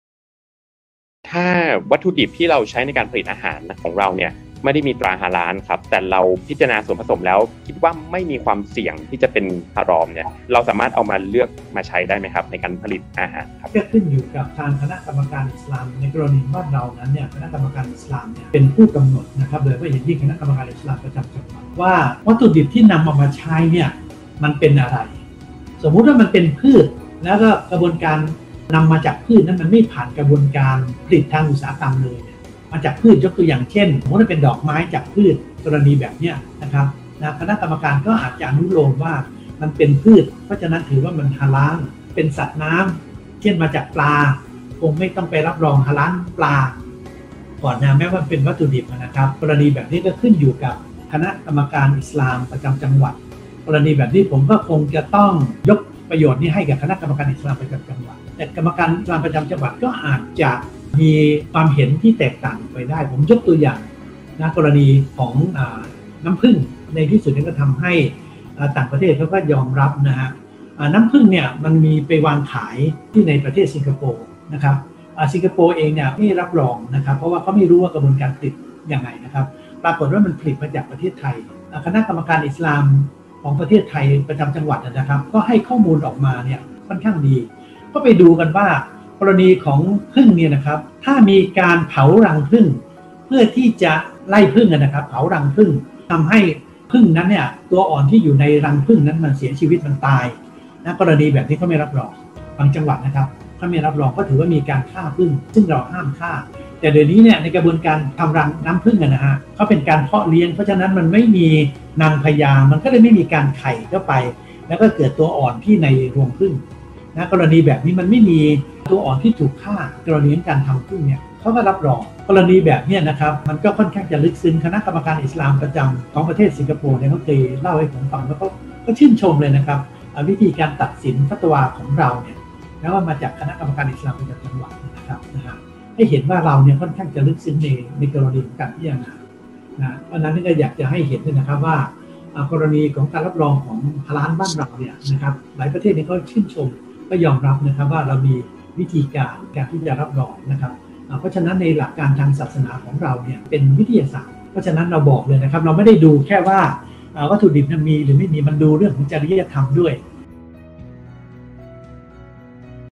THA Academy EP. 06 ฮาลาลจากหลักศาสนาสู่มาตรฐานเชิงวิทยาศาสตร์ 3 Min
"ฮาลาลจากหลักศาสนาสู่มาตรฐานเชิงวิทยาศาสตร์”
โดย รศ.ดร.วินัย ดะห์ลัน
ผู้อำนวยการศูนย์วิทยาศาสตร์ฮาลาล จุฬาลงกรณ์มหาวิทยาลัย
วันพุธ ที่ 30 มิถุนายน 2564 เวลา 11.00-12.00 น.
ผ่านช่องทางออนไลน์ Zoom-Webinar

🔴 Live ผ่านเพจ
ศูนย์วิทยาศาสตร์ฮาลาลจุฬาลงกรณ์มหาวิทยาลัย
Drwinai dahlan
YouTube HSC CHANNEL

จัดโดย THA Academy